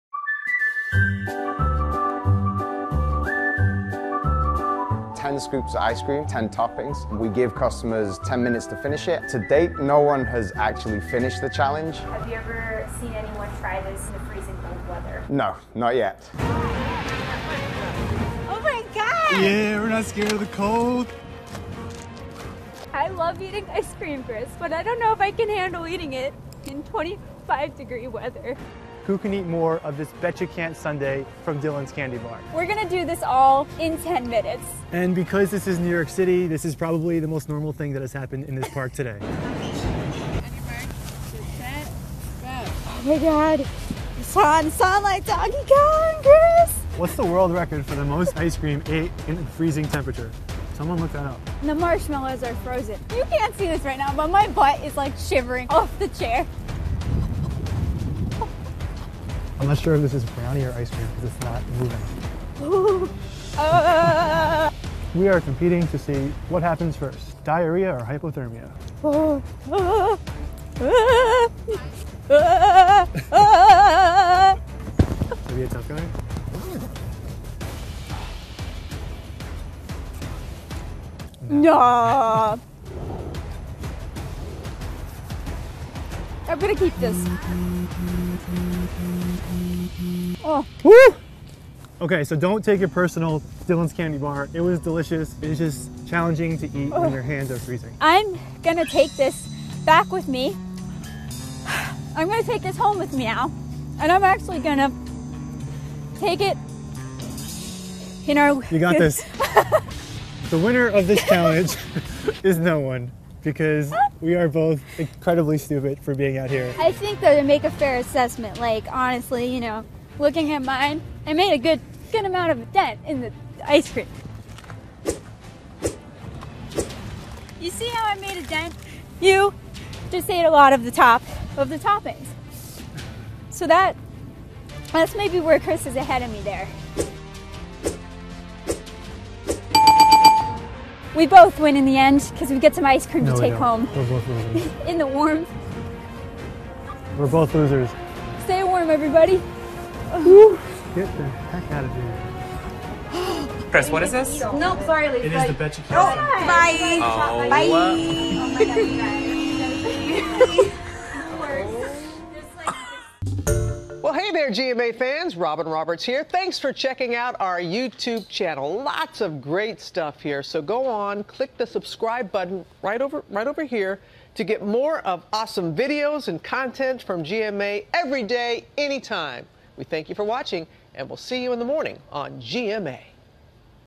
We tried eating this massive ice cream sundae in freezing weather l GMA Digital
We tried eating 10 scoops of the "Bet You Can't" sundae from Dylan's Candy Bar in freezing cold.